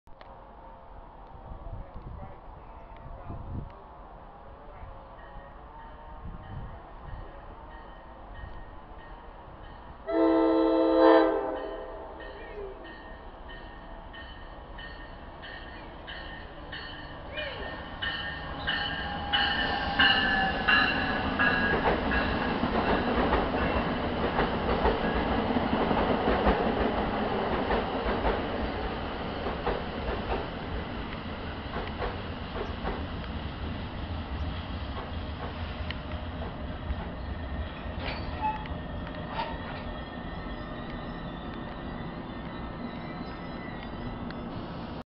NJT Train #3849
Pulling into Princeton Junction Station 11 minutes late at 3:33 p.m. while heading to Hamilton, Trenton & Morrisville Yard with Bombardier Bi-Level Cab Car #7018 in the lead. ALP-46 #4617 is providing power on the rear.